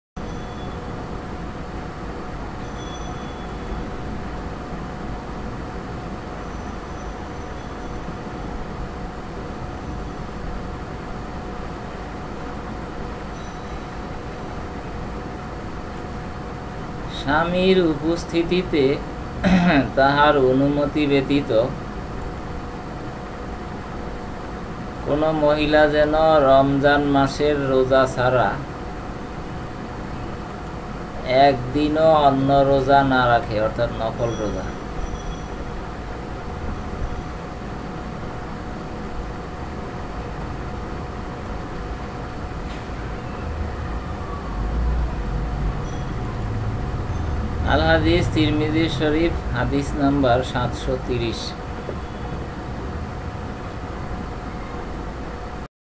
কোন মহিলা যেন স্বামীর উপস্থিতিতে তাহার অনুমতি ছাড়া রমজানের রোজা ছাড়া অন্য রোজা না রাখে
কোন  মহিলা  যেন  স্বামীর  উপস্থিতিতে  তাহার  অনুমতি  ছাড়া  রমজানের  রোজা  ছা